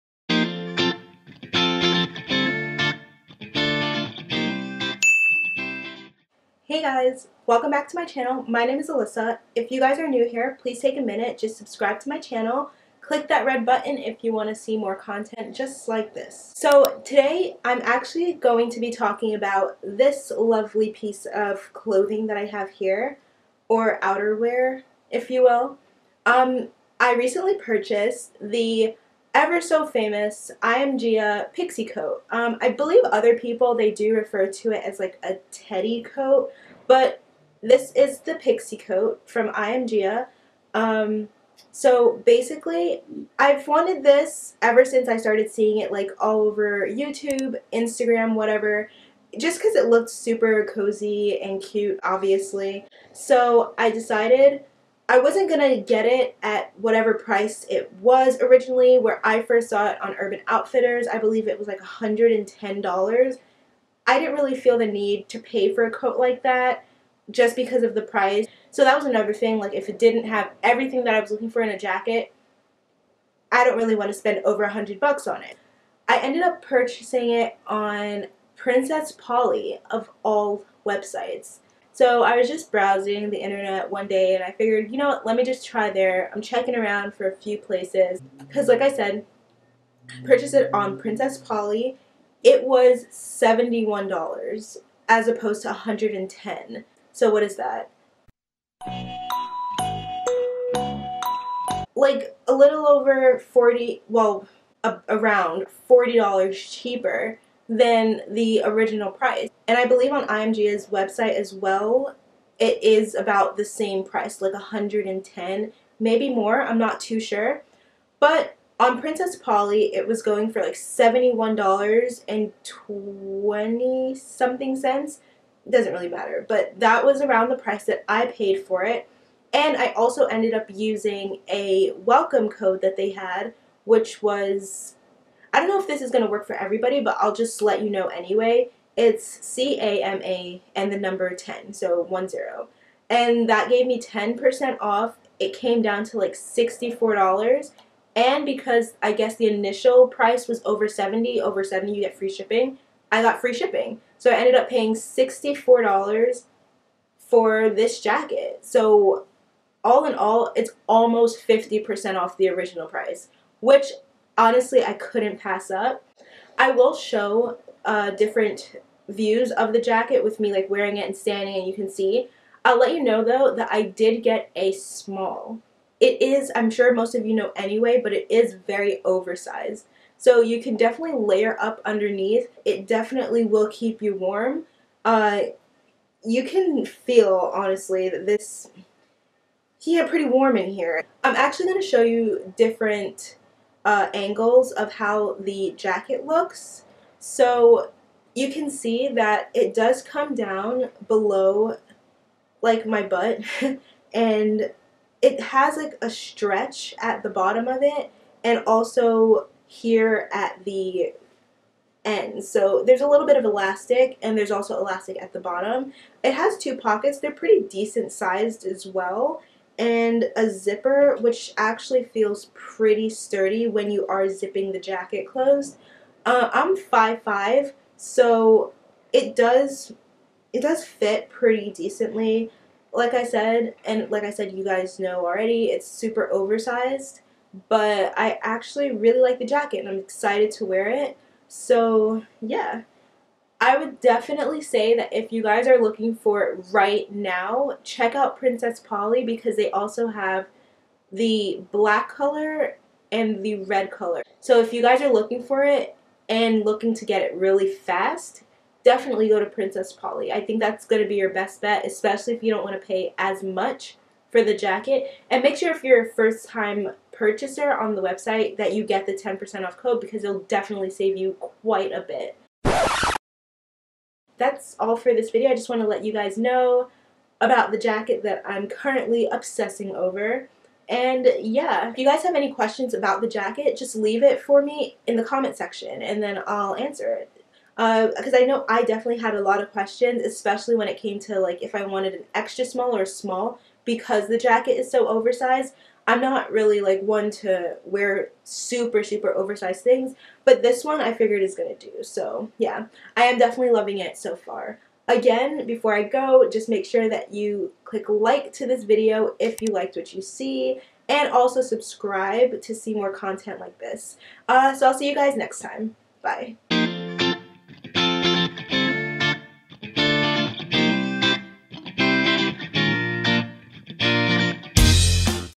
I.AM.GIA PIXIE COAT REVIEW
HIYA bubble butts. Long time no see 🙈 I decided to do my own review of the famous IAMGIA Pixie coat that became super popular last year i think.